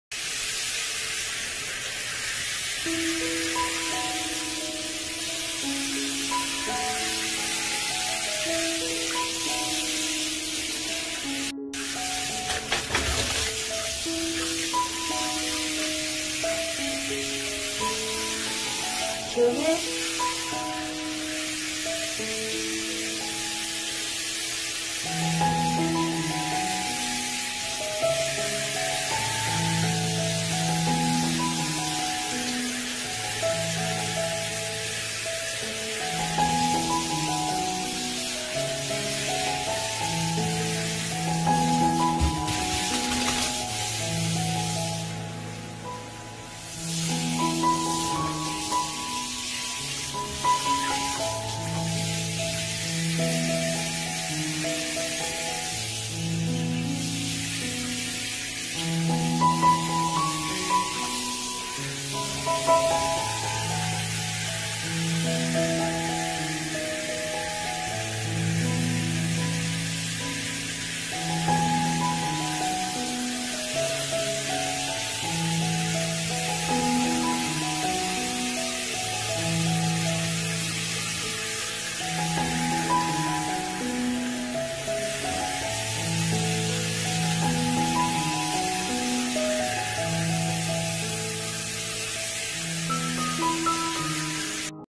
강아지🐶 말티푸🐕 하니의 피서법 🐕개 시원🐕 하니랑 산책후 샤워.목욕.시원하다는듯 즐기는것 같아요☆☆☆
은근 샤워를 즐기는 느낌
좋아해서 더울때
샤워 물놀이☆☆☆☆☆